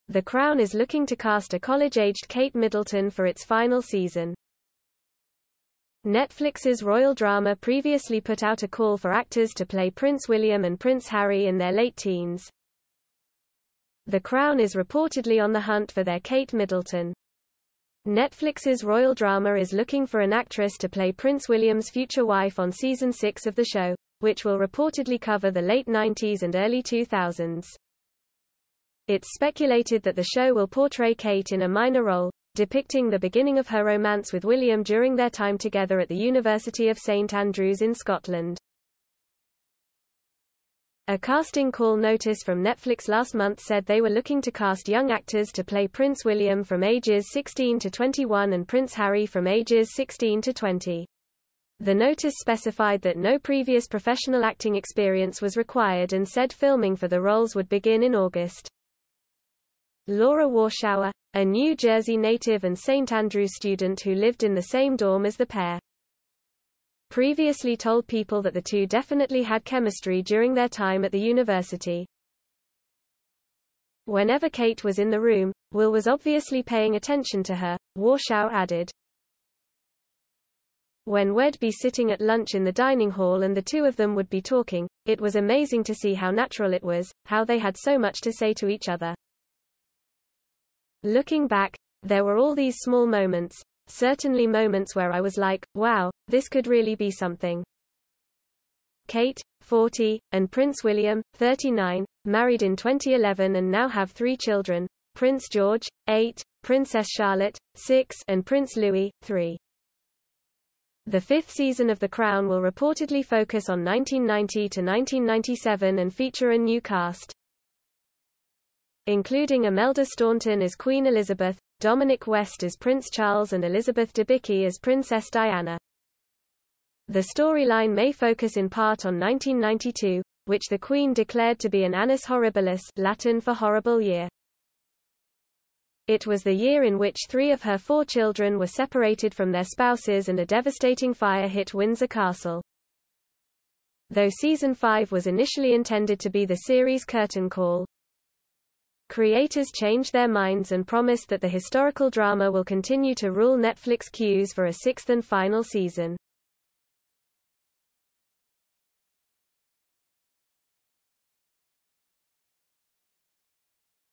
The Crown Is Looking to Cast a College Aged Kate Middleton for Its FinaI Season
Netflix's royal drama previously put out a call for actors to play Prince William and Prince Harry in their late teens